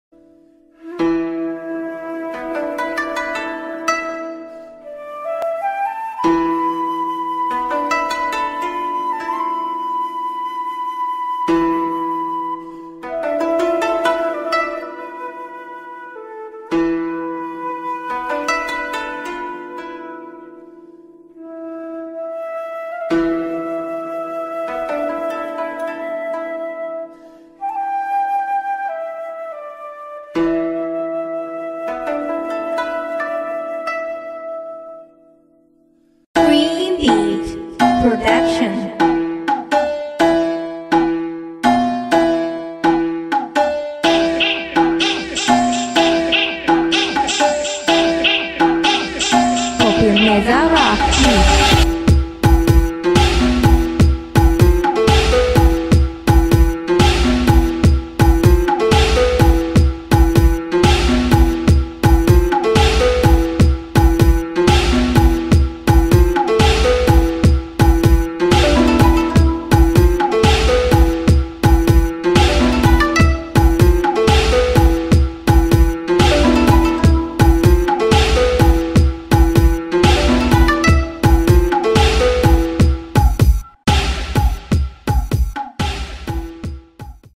VOGUE SPRING - NEW CHOREO BY VARVARA NAYNISH
Продолжаем развивать Японские мотивы и любознательность на workshopsbybarbara, ибо когда есть choreoday и немного naynishchoreo = получается такой свежий бомба-микс 🔥
Choreo
Place: @ekb.tancy.pro
Dancers: @_sasha_iv @baturina_olya barbarasteam снова в деле)